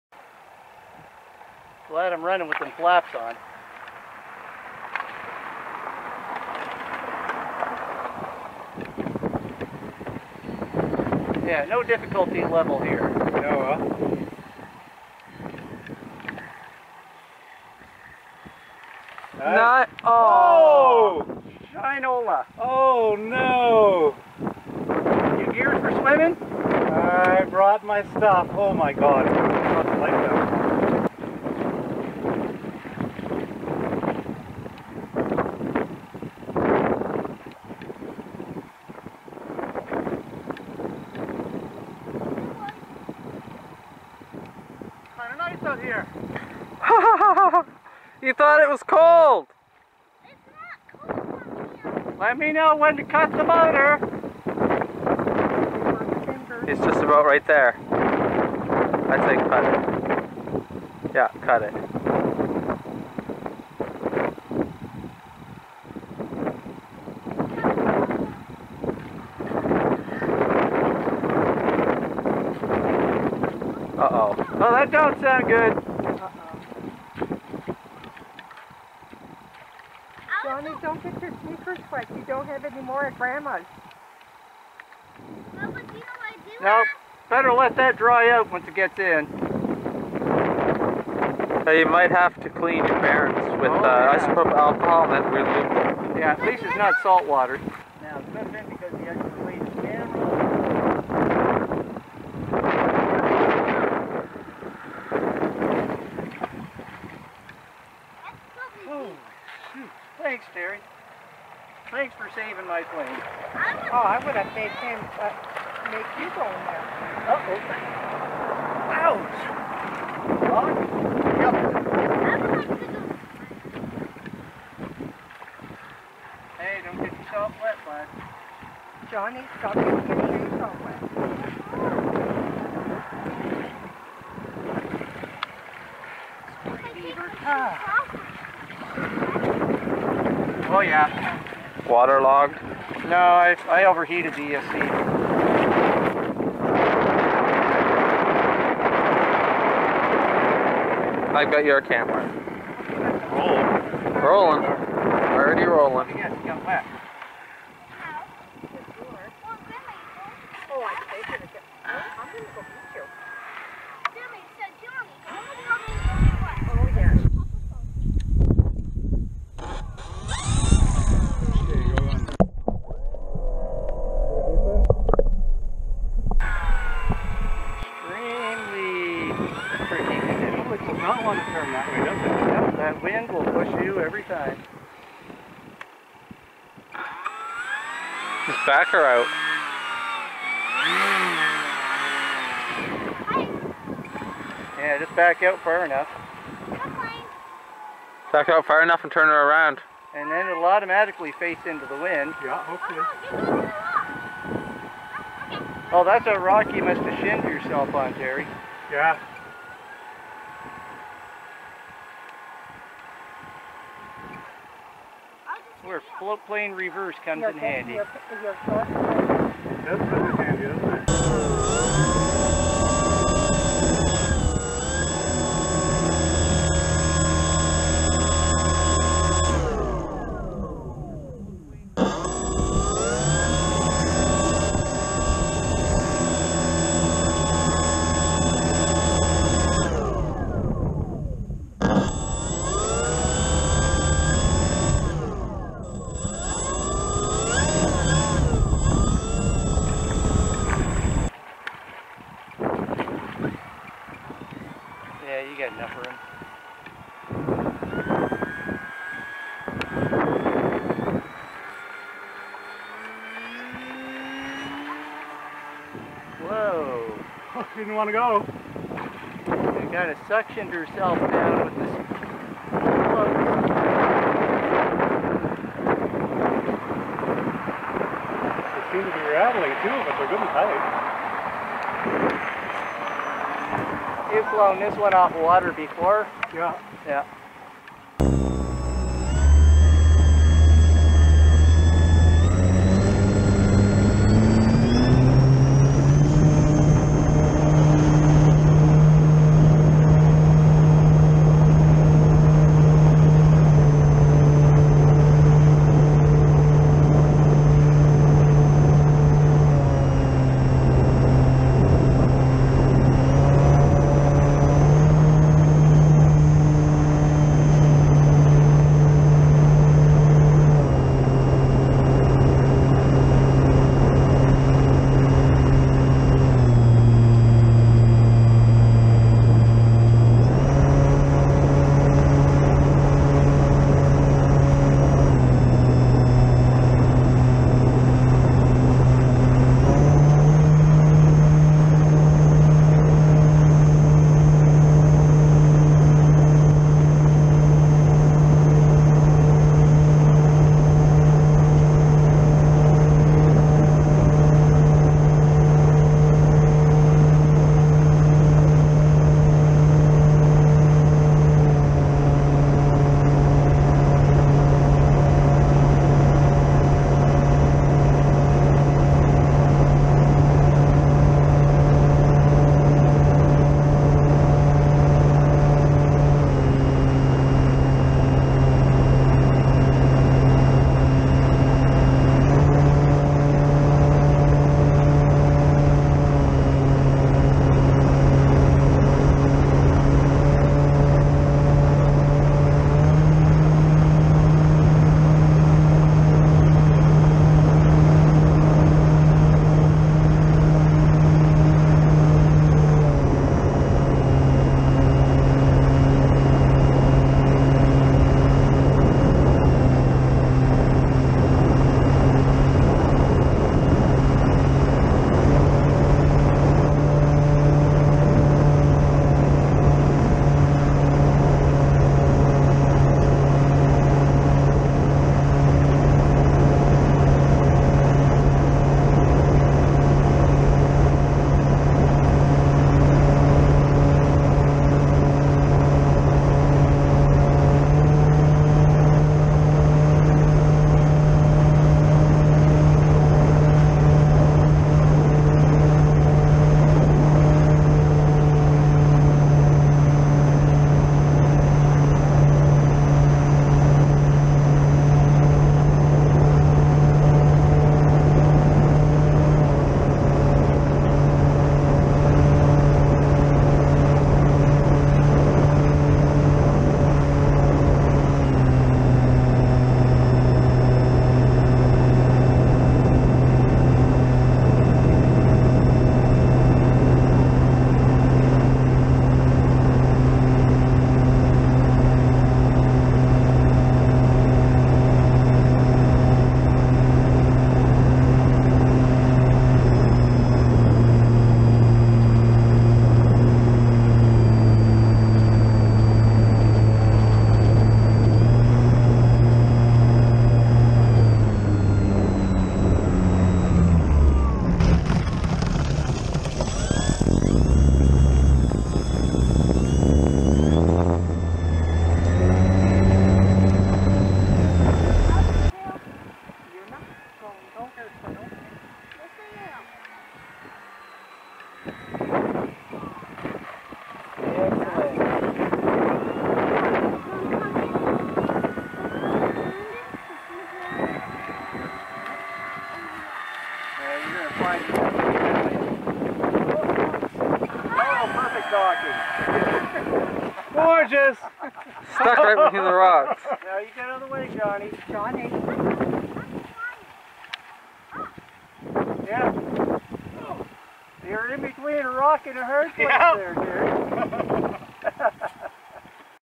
Durafly Tundra Float Cam
The Hobbyking Durafly Tundra enduring winds that she shouldn't be float flying in, off of Allens Lake on August 5th 2018. Short Beach, Nova Scotia.